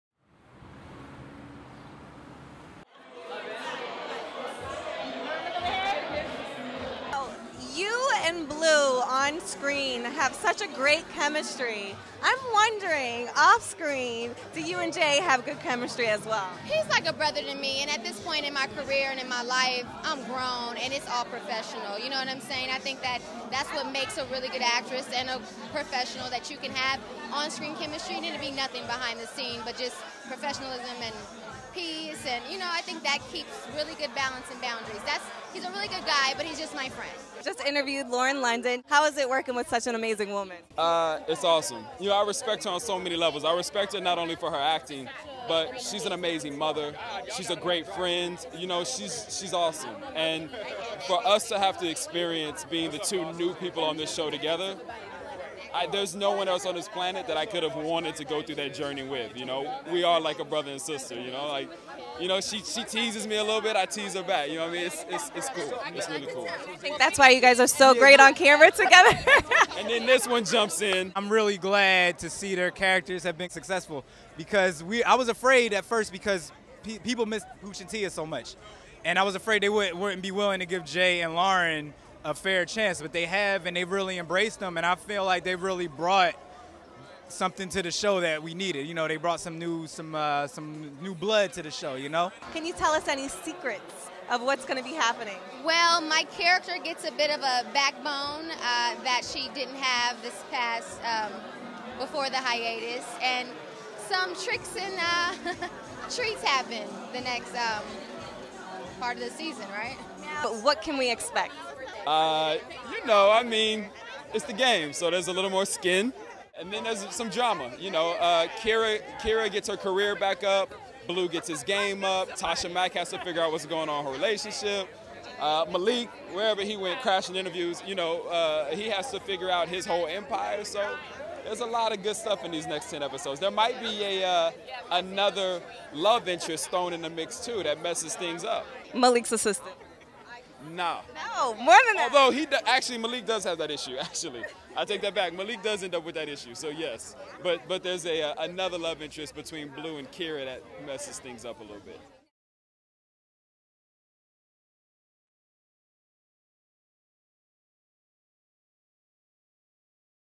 Jay Ellis and Lauren London Dish On Their On-Screen and Off-Screen Chemistry
The Game's newest stars Lauren London and Jay Ellis chat on what it's like to work together and if their on-screen chemistry translates off screen.  Barry Floyd also makes an appearance